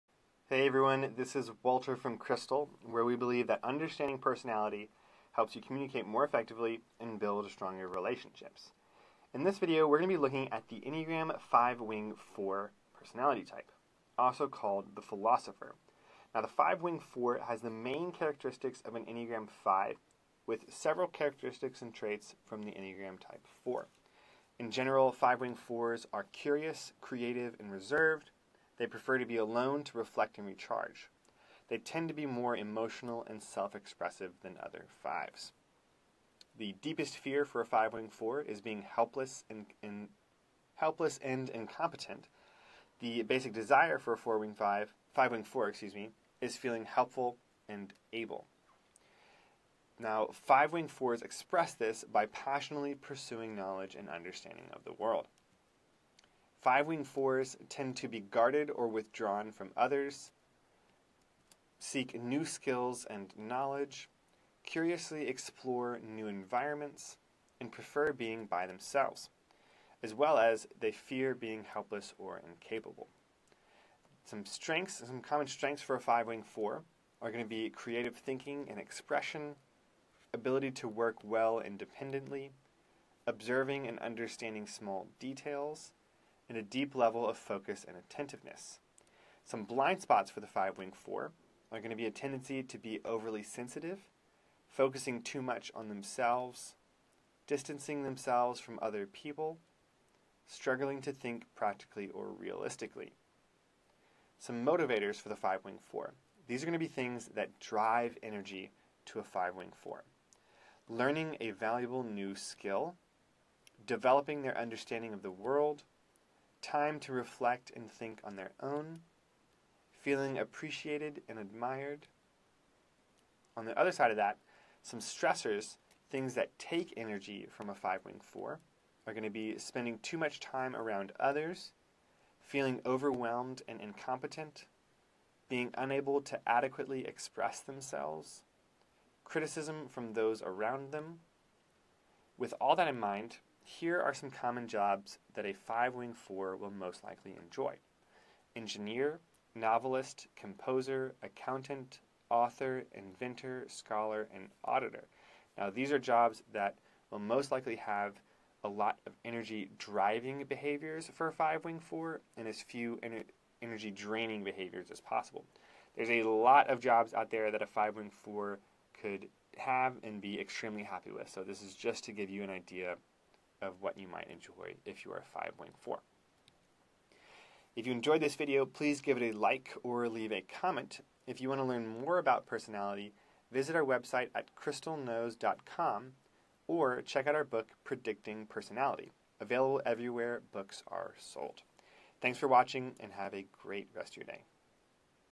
Enneagram 5w4 Personality Type
This video will help you understand Enneagram 5 Wing 4 personality types. Understand their deepest fears and desires, what their strengths, blindspots, motivators, and stressors are, and a few common jobs for a 5 Wing 4.